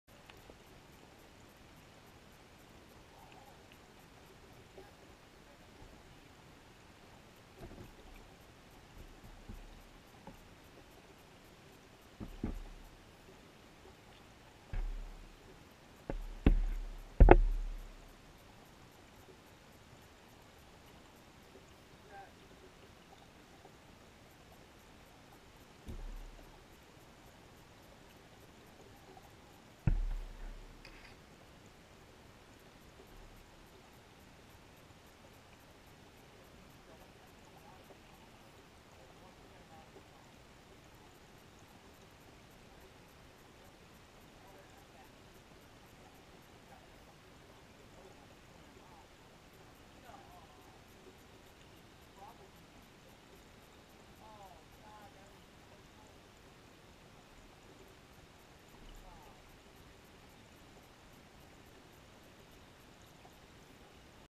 Minnow Escapes from Muskellunge Gullet - Musky Feeding Frenzy
My muskellunge(musky) catches a minnow and is holding it dog bone style. If you watch, as the musky tries to swallow the minnow in it's mouth, another minnow just swims out of the musky's mouth. For a second you can see the musky holding both minnows in his teeth. Please Like if you do.